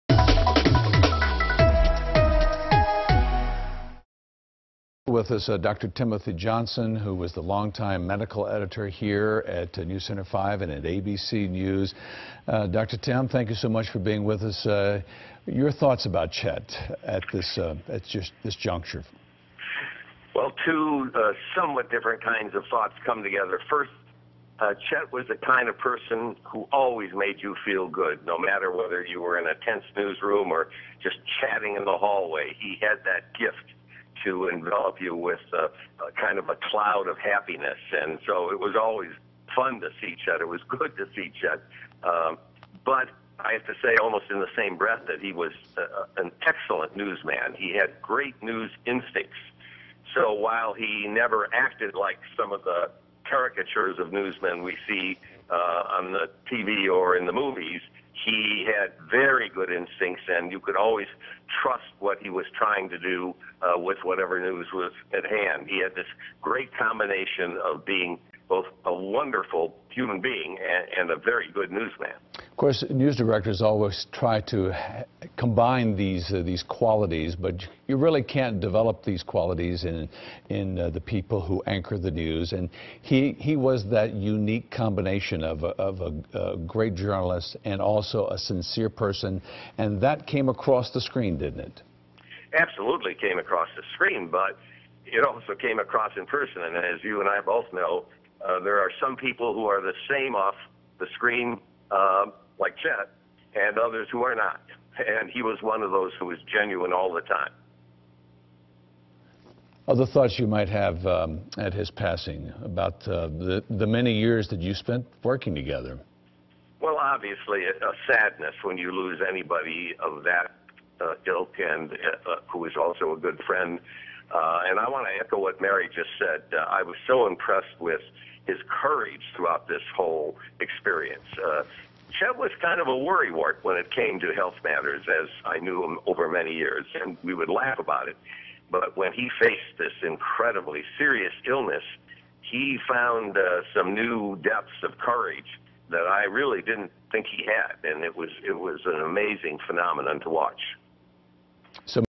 Dr. Tim Johnson: Chet always made you feel good
Dr. Tim Johnson remember's Chet Curtis's ability to "envelop you in happiness" and make you feel good.